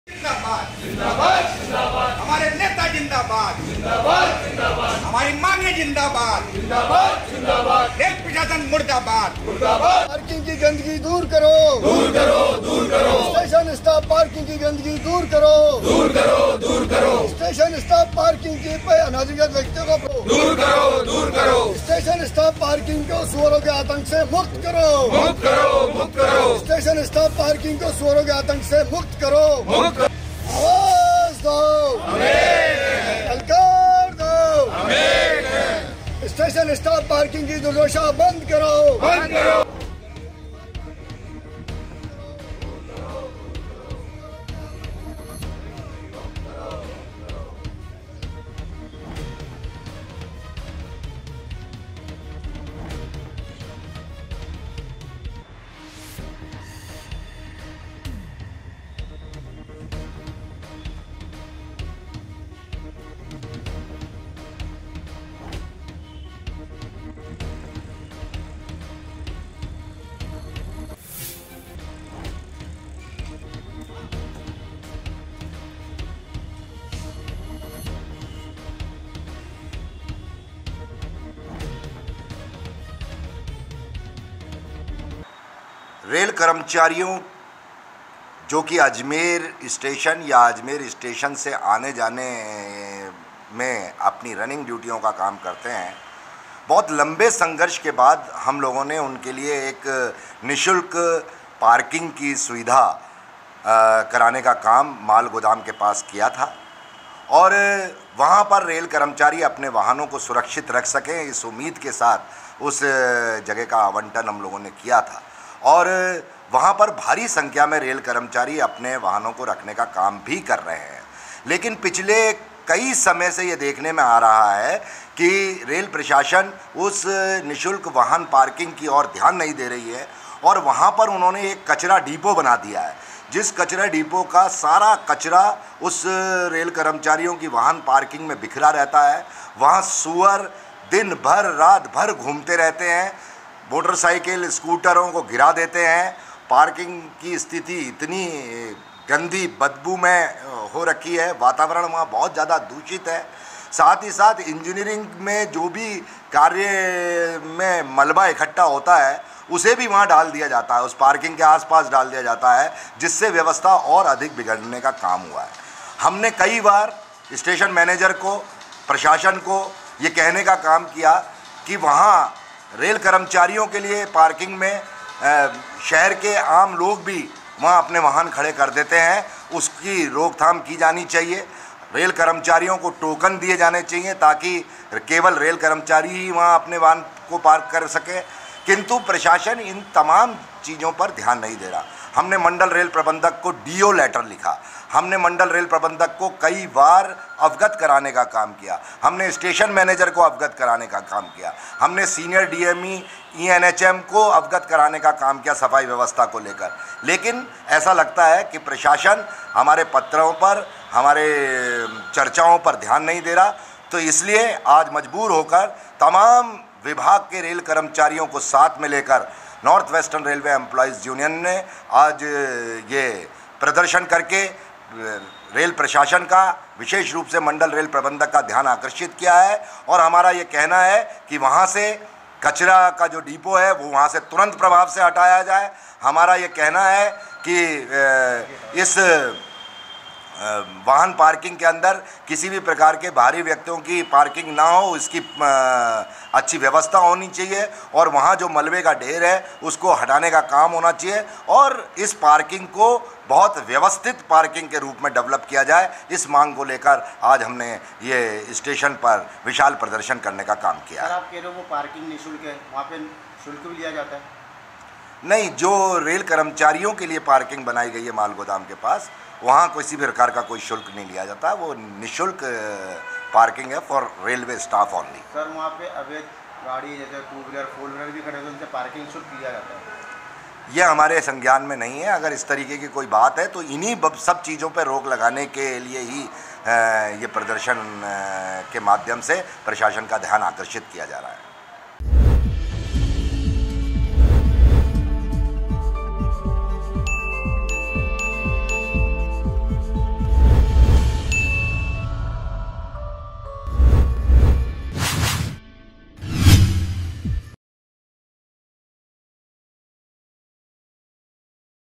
AJMER NEWS | नॉर्थ वेस्टर्न रेलवे एंप्लाइज यूनियन ने किया विरोध प्रदर्शन | MTTV INDIA
नॉर्थ वेस्टर्न रेलवे एंप्लाइज यूनियन ने किया विरोध प्रदर्शन
रेल प्रशासन की लापरवाही की वजह से पार्किंग बनी कचरा पात्र स्थल
रेलवे कर्मचारियों के लिए निशुल्क माल गोदाम के पास पार्किंग का हुआ था आवंटन
यूनियन ने पार्किंग व्यवस्था को उचित व्यवस्थित और स्वच्छ किए जाने की माँग की